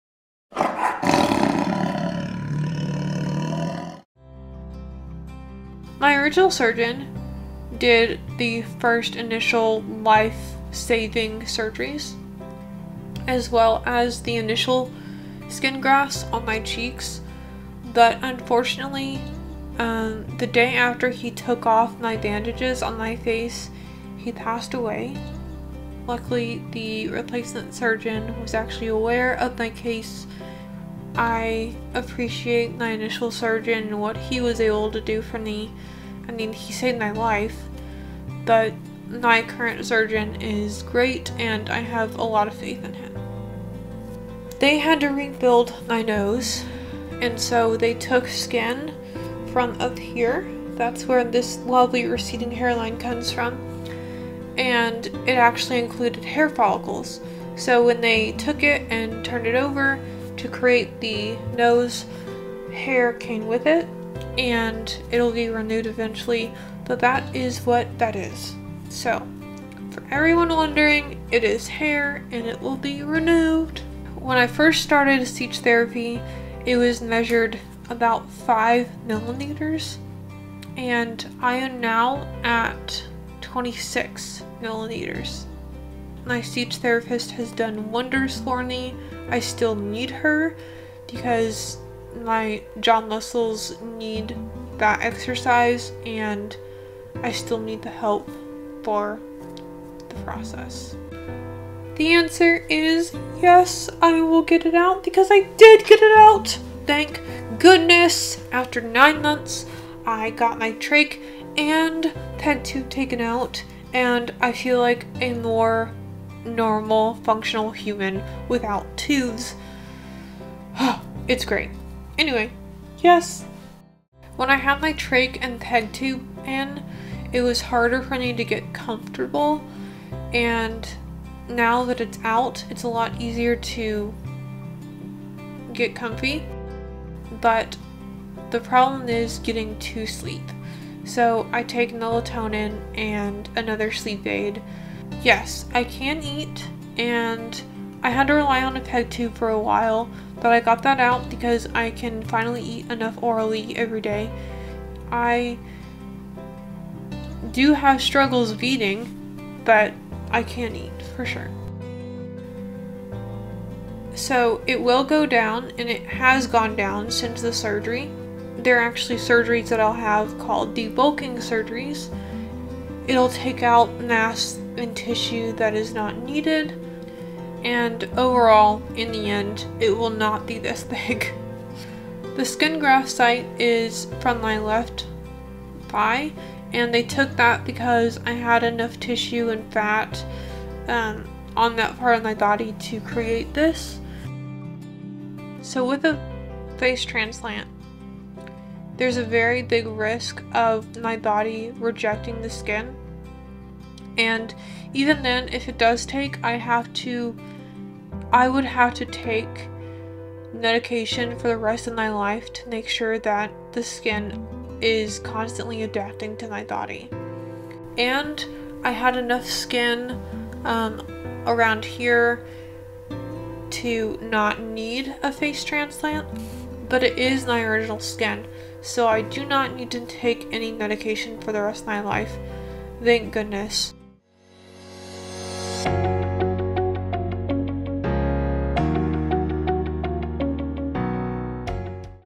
Recovery FAQ Video Part Two
Part two is here! More questions regarding my recovery process are answered in this video. **Captions are available**